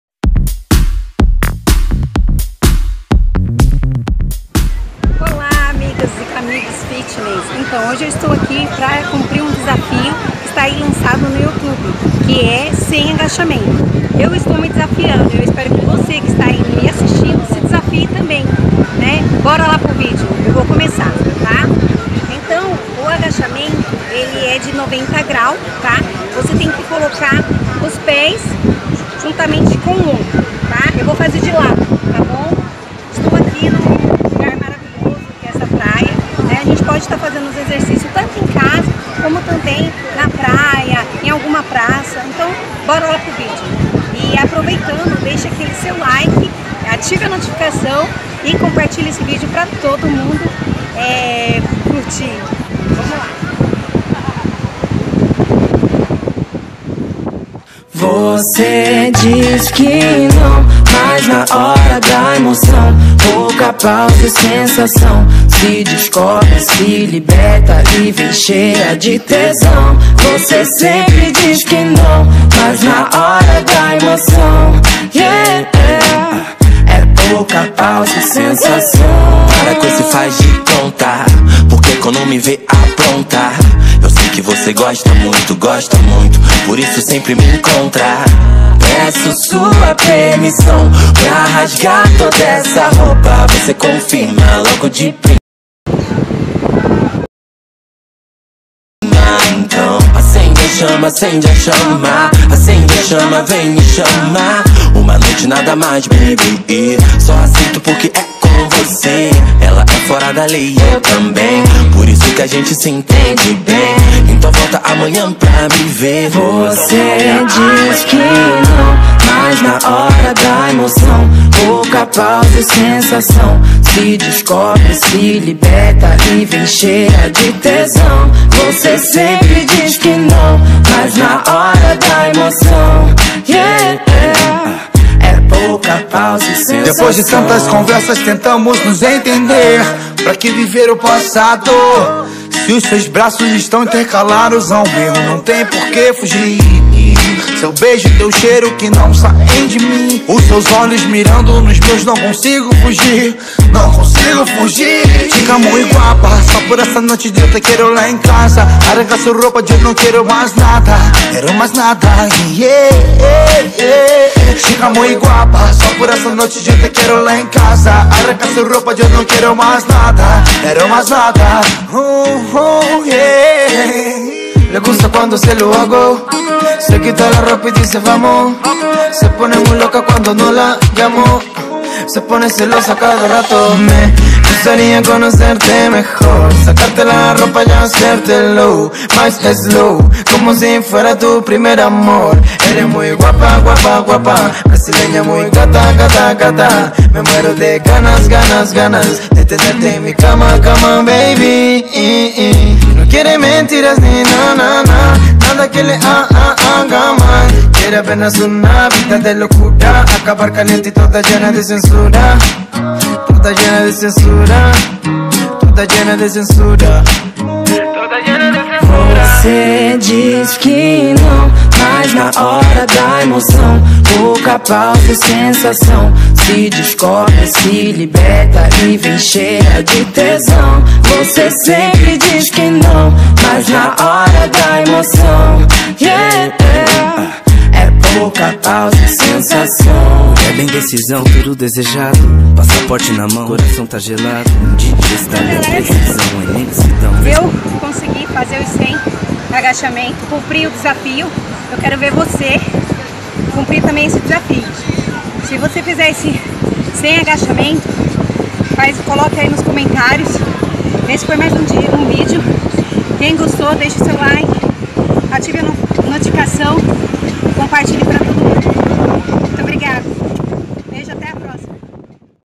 Treino em casa Desafio 100 Agachamento💪💪💪
Será que ela consegui 😂🤔
Você já fez o seu poste aí e que vou vê e responder😉
Me sigam lá no Instagram Katiaemine👍